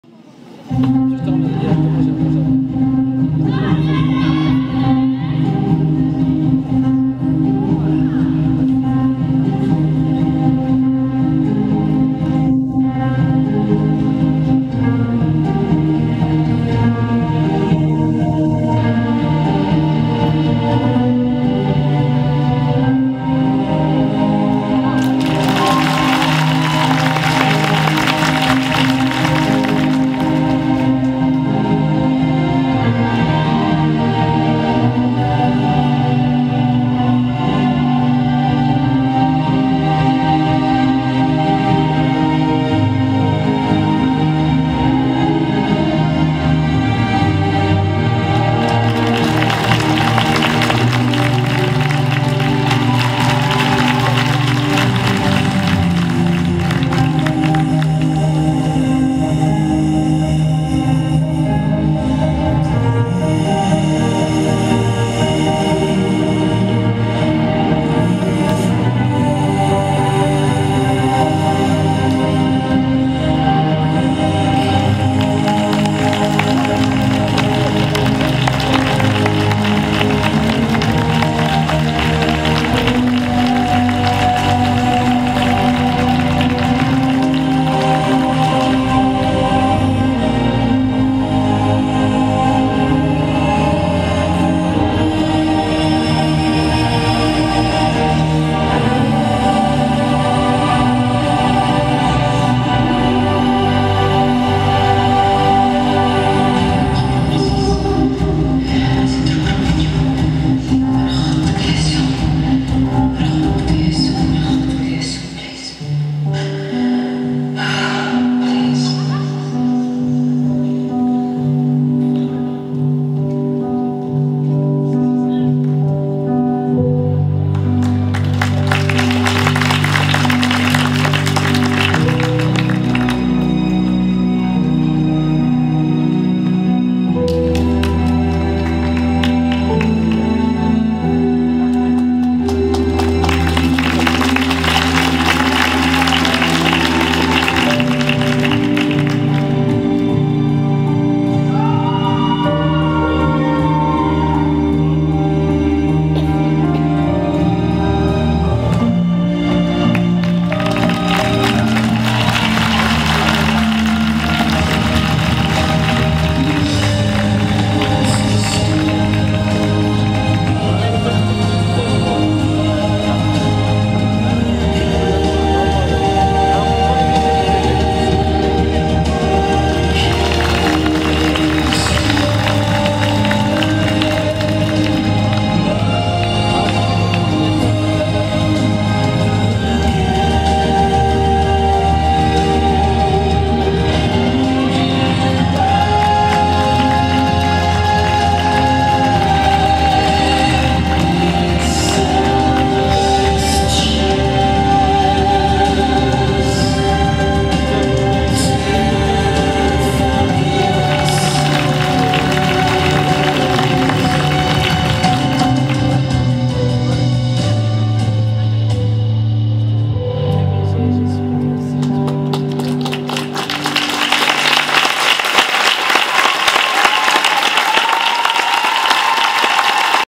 La fête du Patin 2022: Club Patiner à Plouguin : Solo Alexandre
Le solo d'Alexandre du Club Patiner à Plouguin